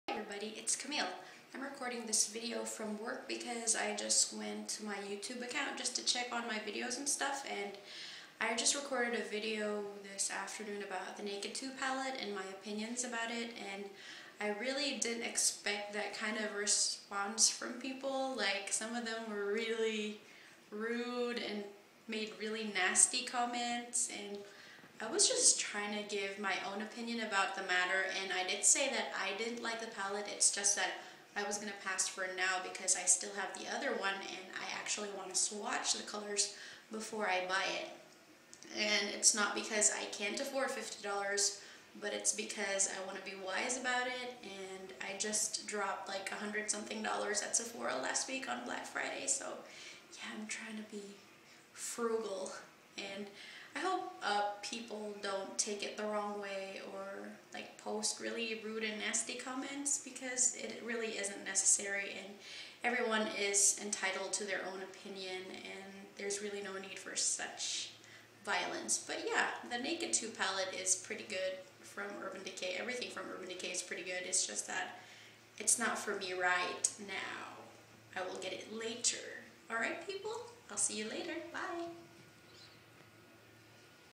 Response to Passing on Urban Decay 2 Palette
Be nice people.. There is no need to be rude or nasty :) just expressing myself :) If you don't hav anything nice to say, don't say it :)